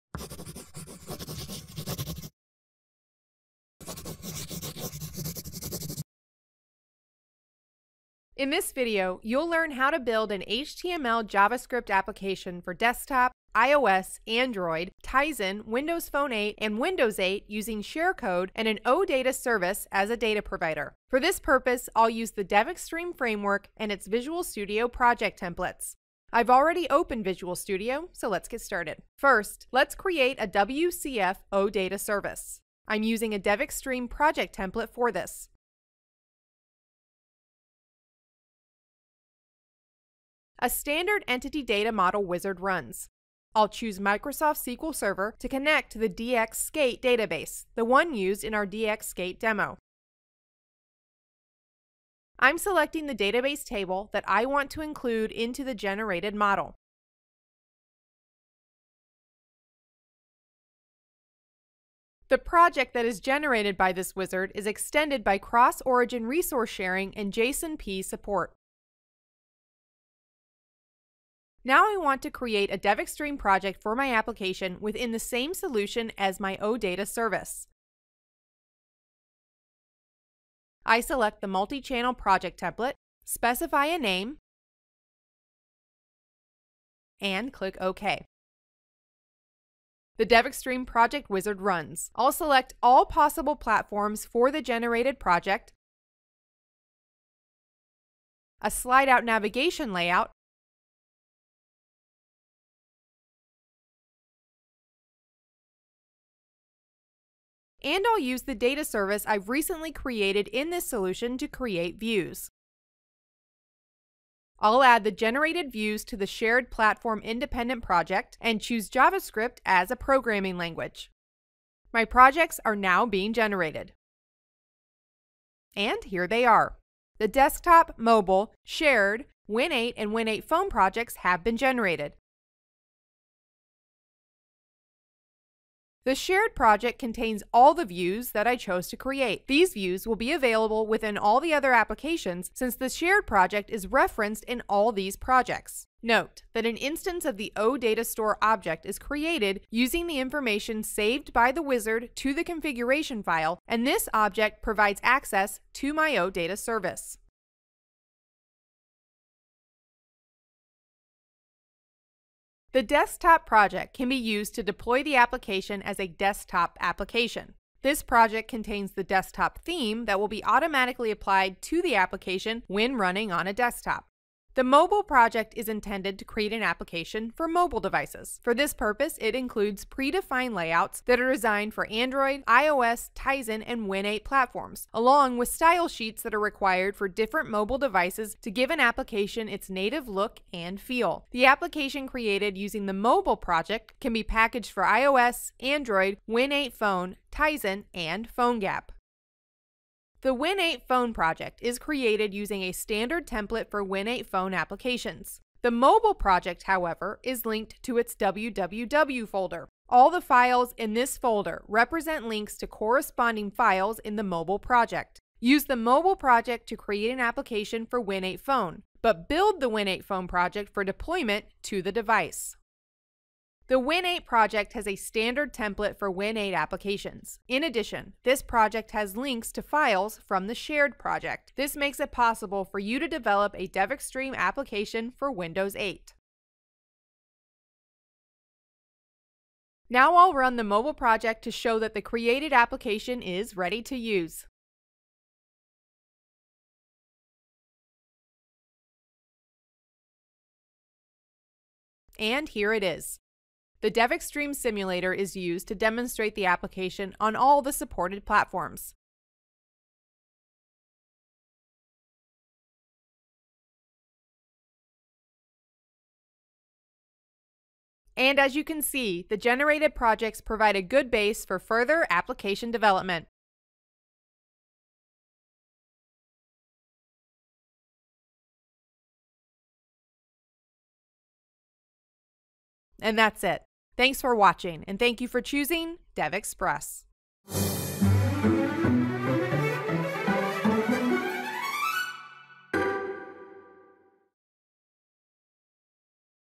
DevExpress DevExtreme: Creating a Multi-Channel App
Learn how use DevExtreme to build an HTML JS application for desktop, iOS, Android, Tizen, WindowsPhone8 and Windows8 platforms using a shared code and an OData service as a data provider.